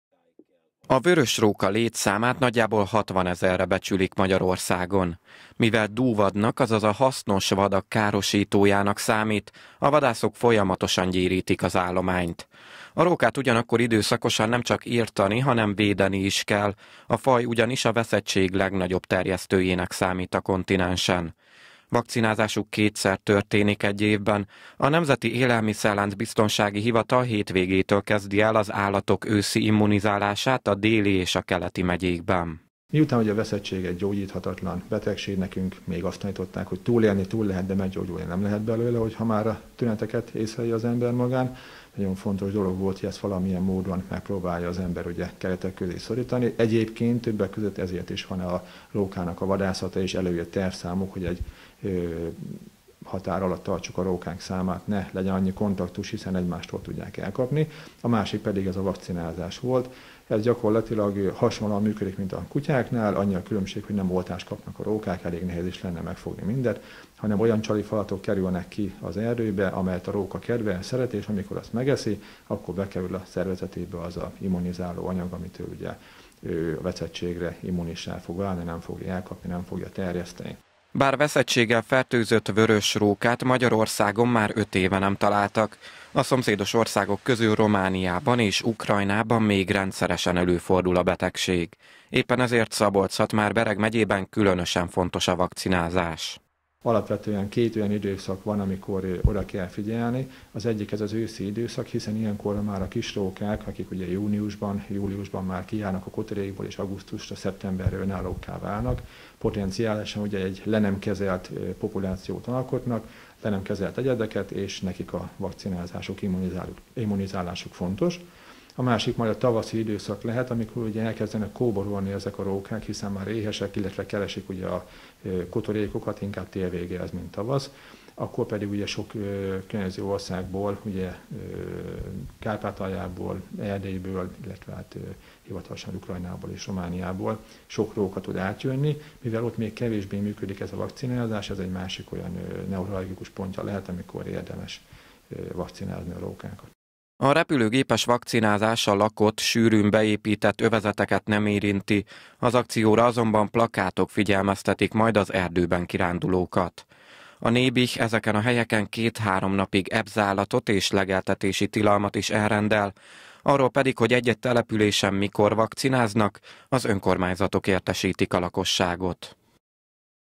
Őszi rókavakcinázás
Szombattól kezdi az őszi repülőgépes rókavakcinázást Szabolcs-Szatmár-Bereg megyében a Nemzeti Élelmiszerlánc-biztonsági Hivatal. Az akció elsősorban az erdős területeket érinti majd, ezeken a helyeken akár több napig tartó ebzárlatot és legeltetési tilalmat is elrendelnek. A vörös róka immunizálása egyébként évek óta eredményes, veszett állatot utoljára öt évvel ezelőtt találtak az országban.